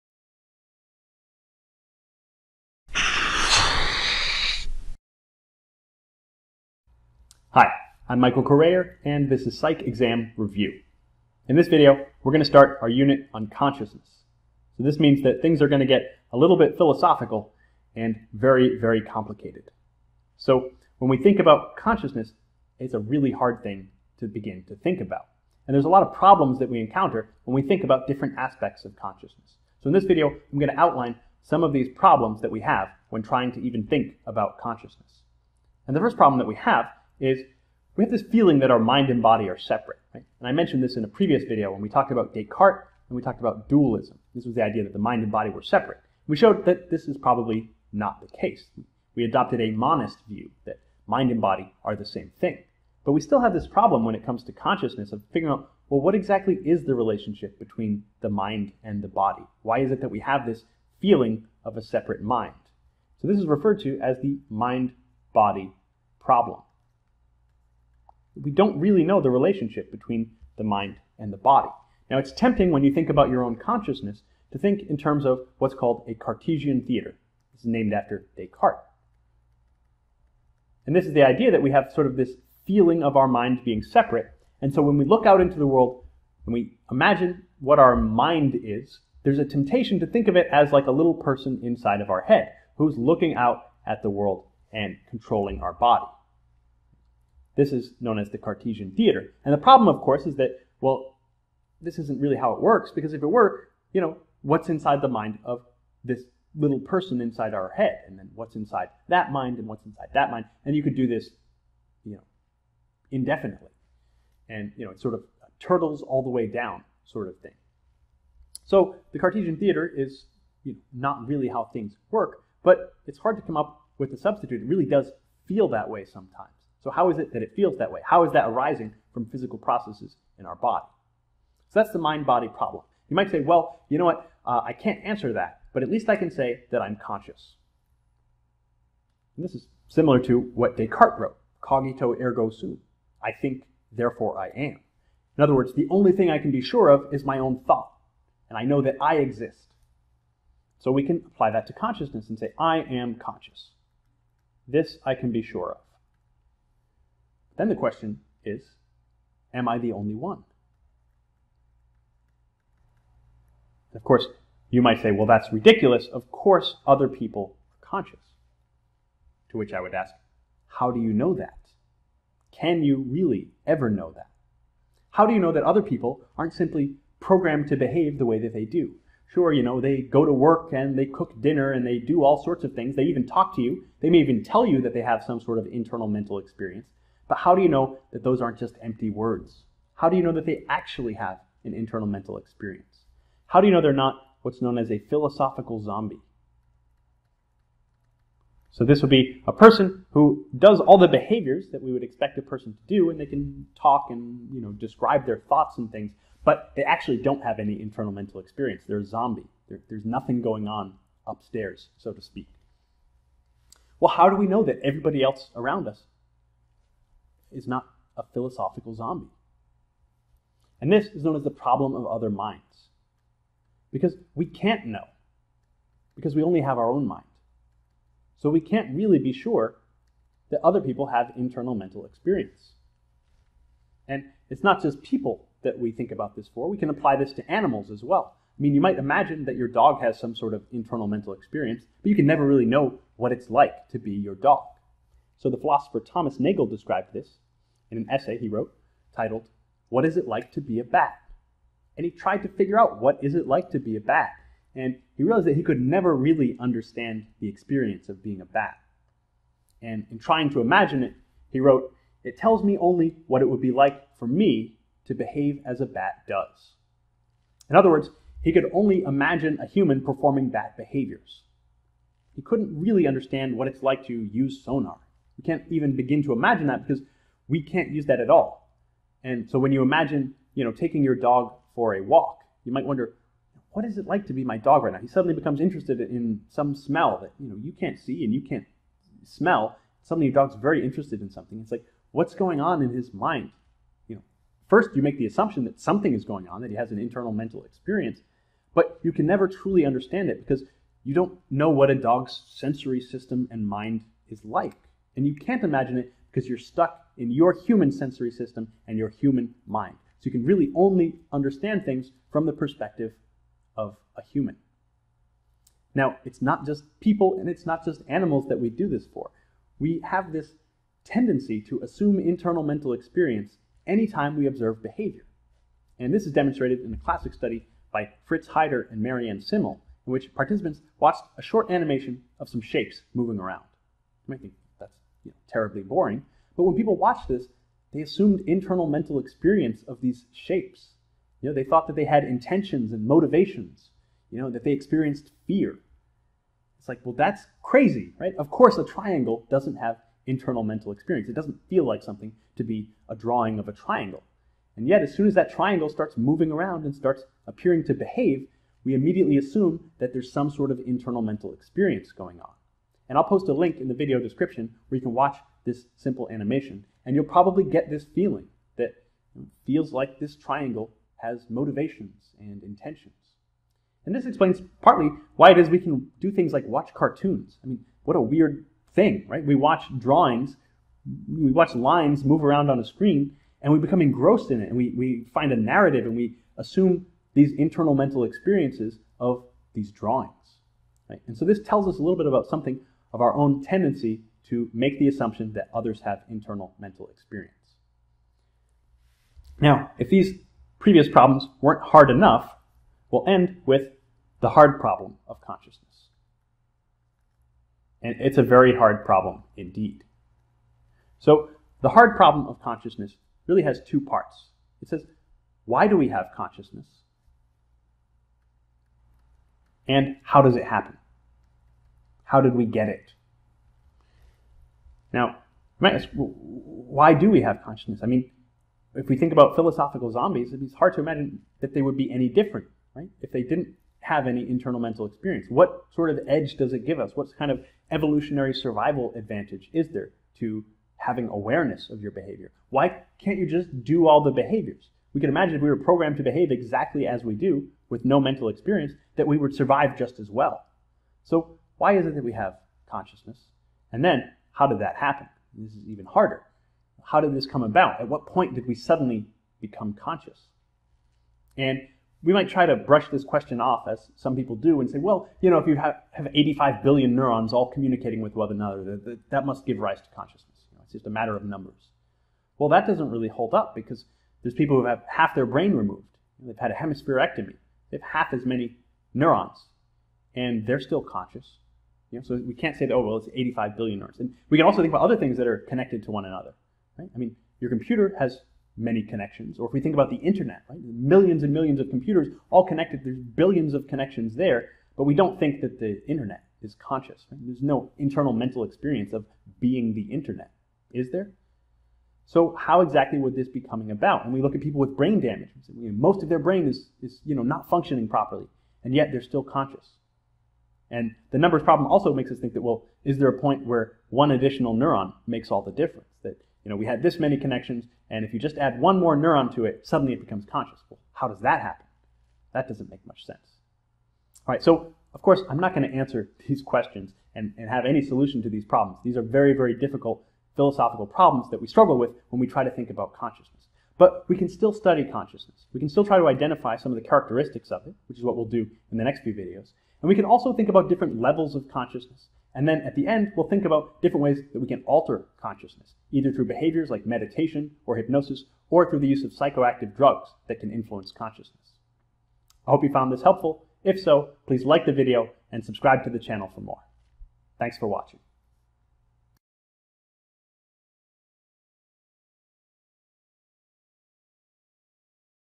Problems of Consciousness (Intro Psych Tutorial #97)
In this video I begin the unit on consciousness by considering a number of problems associated with understanding consciousness. These include the mind-body problem, the problem of other minds, and the hard problem of consciousness. While we don't have solutions to these problems, we can still study certain aspects of consciousness and attempt to understand the characteristics of consciousness, levels of consciousness, and altered states of consciousness.

Check out my psychology guide: Master Introductory Psychology, a low-priced alternative to a traditional textbook:

Thomas Nagel – What is it like to be a bat? (1974)